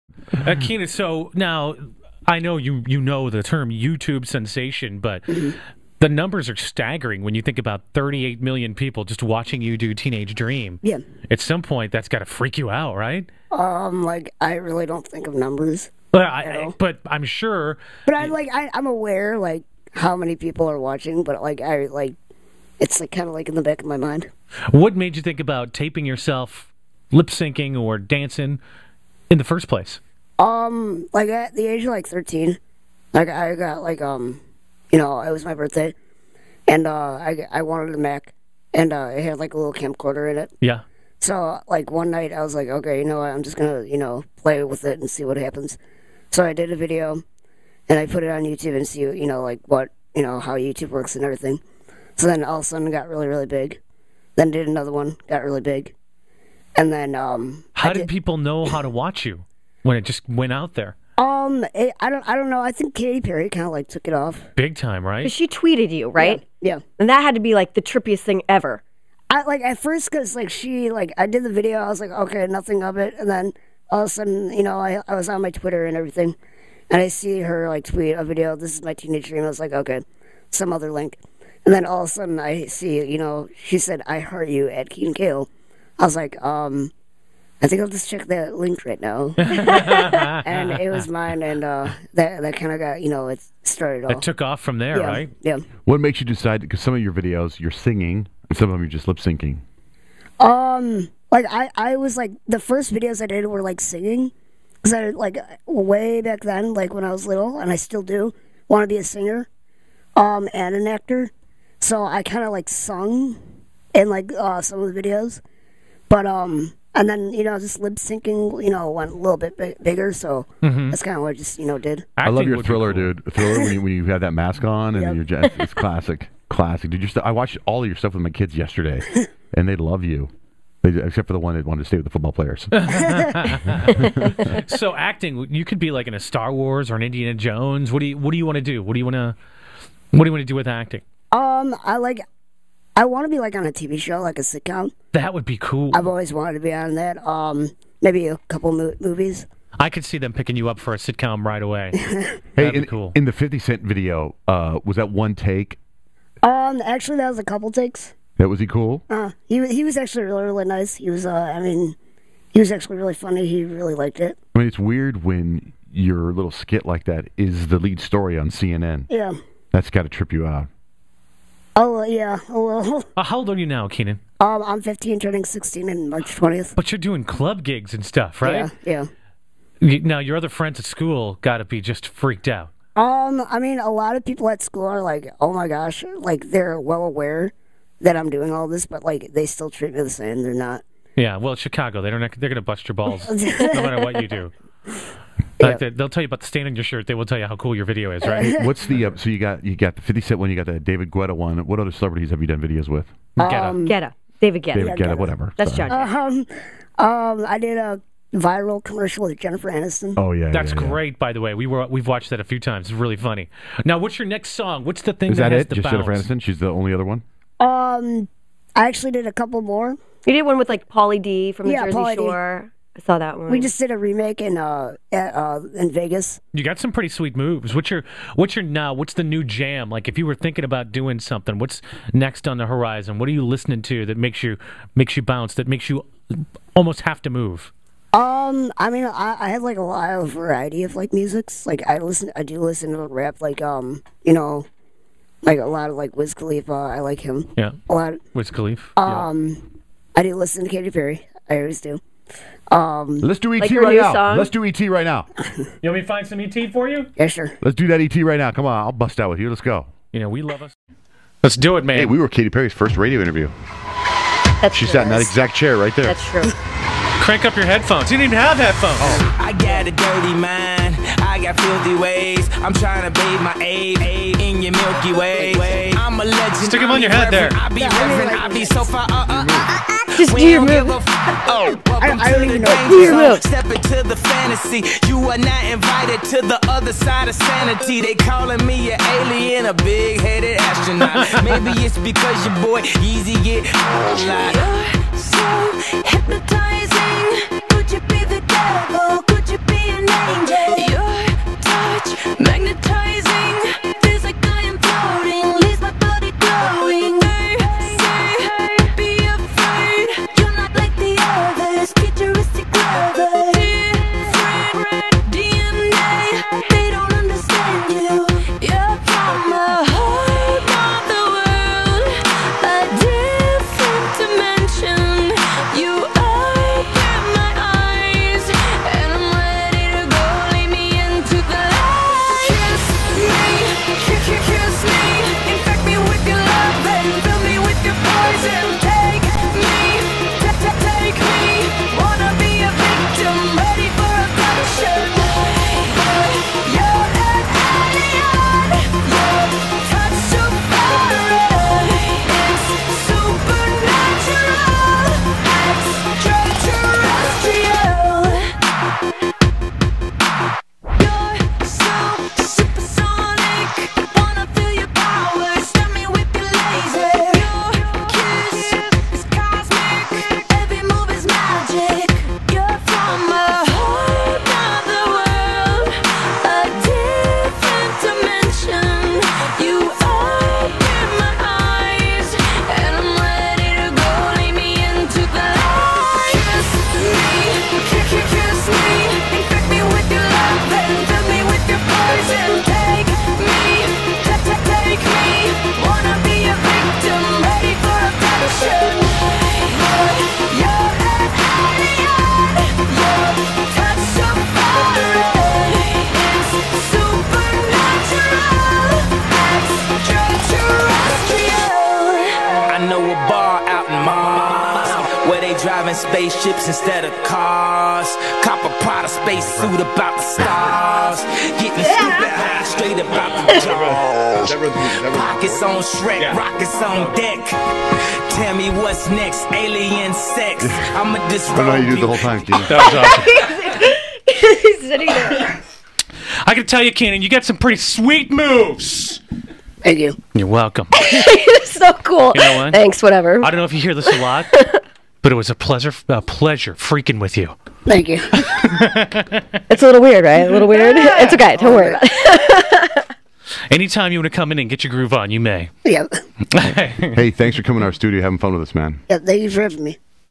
Keenan Cahill In-Studio With Johnjay & Rich
YouTube sensation Keenan Cahill dropped by the studio and jammed out with the crew!